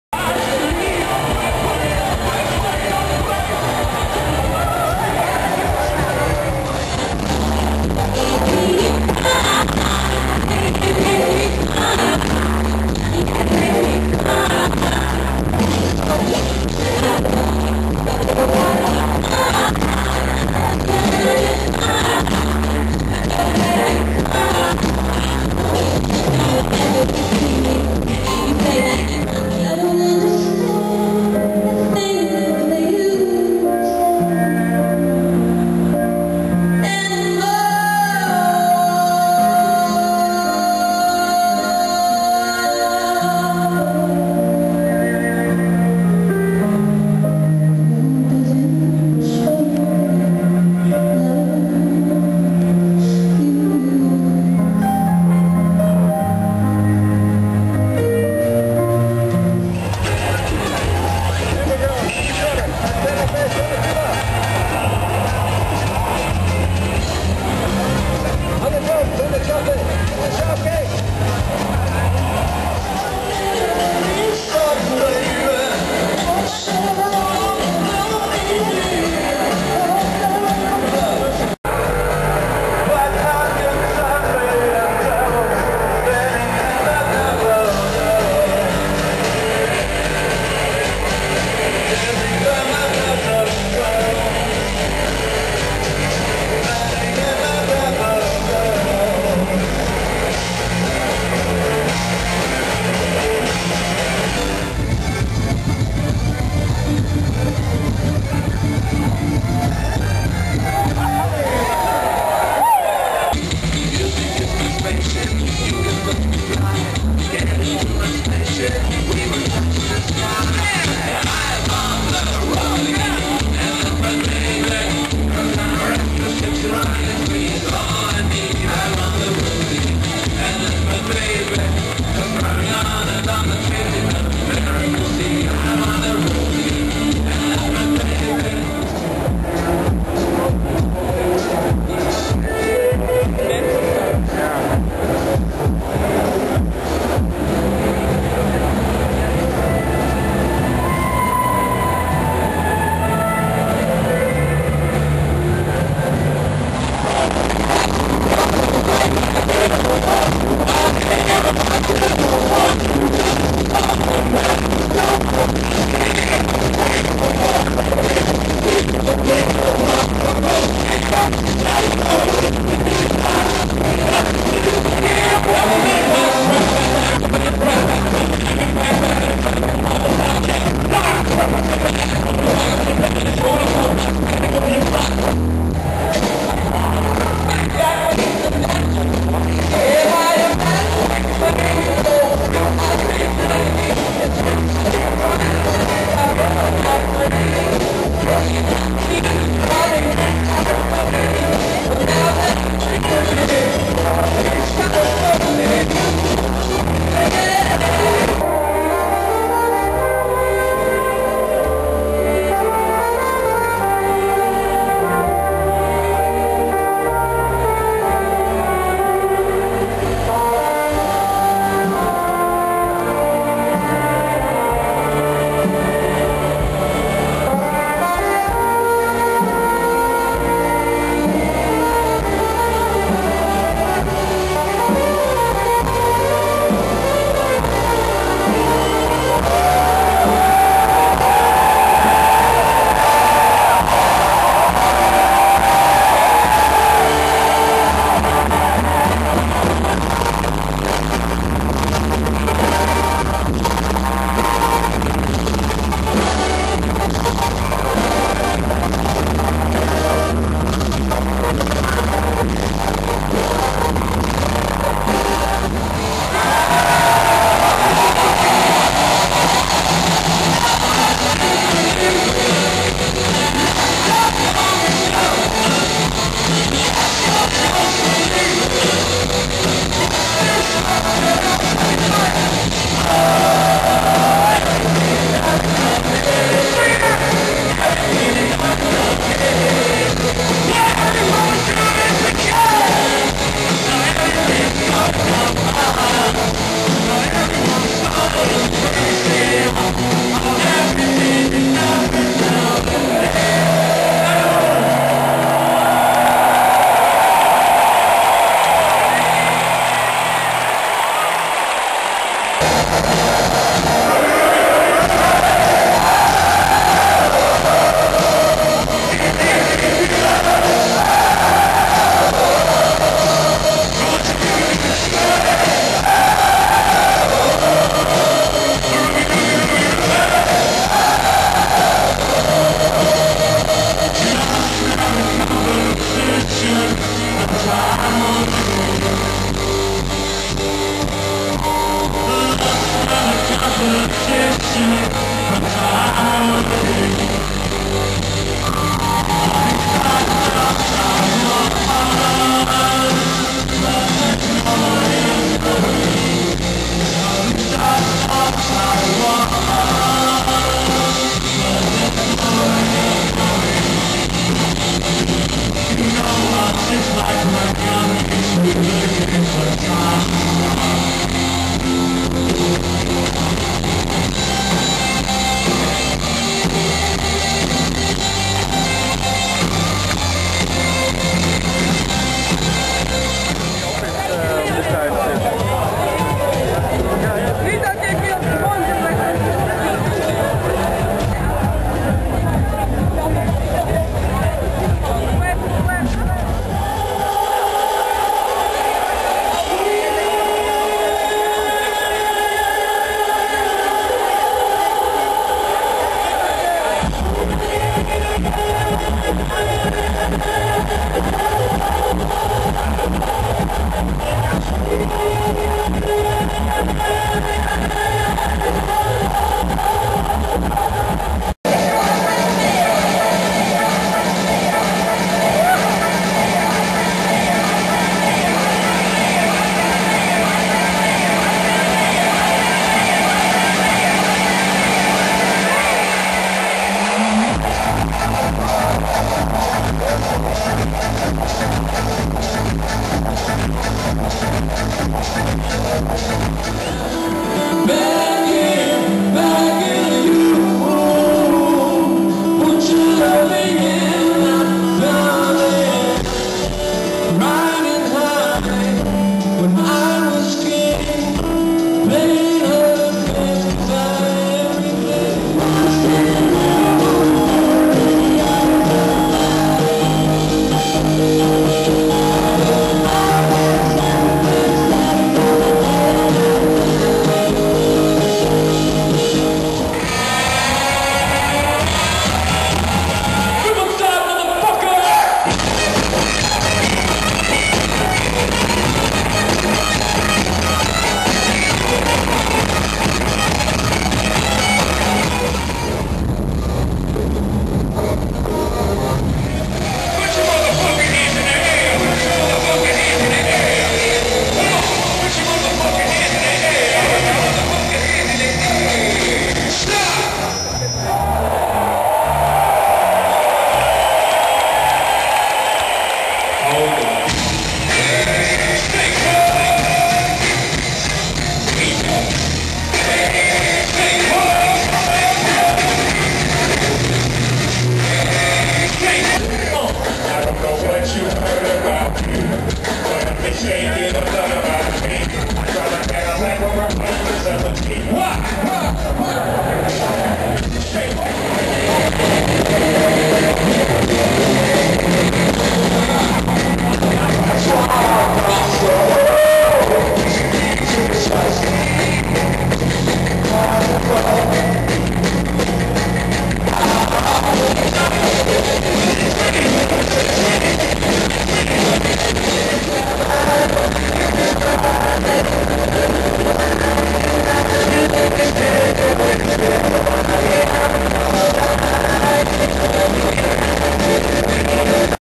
Compilation Lowlands Festival 2009
Just to give you an impression of the festival. I can assure you the sound is much better in real life :P It was very hard to hold the camera steady with so many enthousiastic people on site!